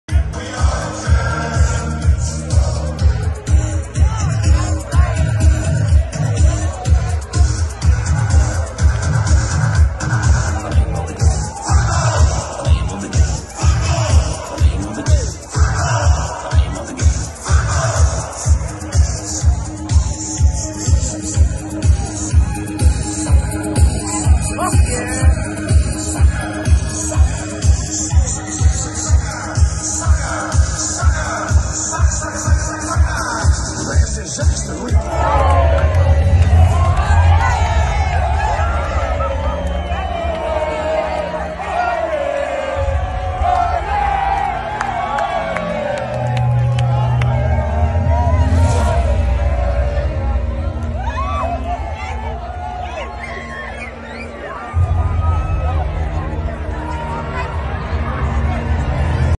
Spirited atmosphere as Bokke well supported at Flying Pig fan park
The Flying Pig was the place to be this past Saturday as hundreds flocked in to the fan park to watch the Springboks take on England in the Rugby World Cup semi-final.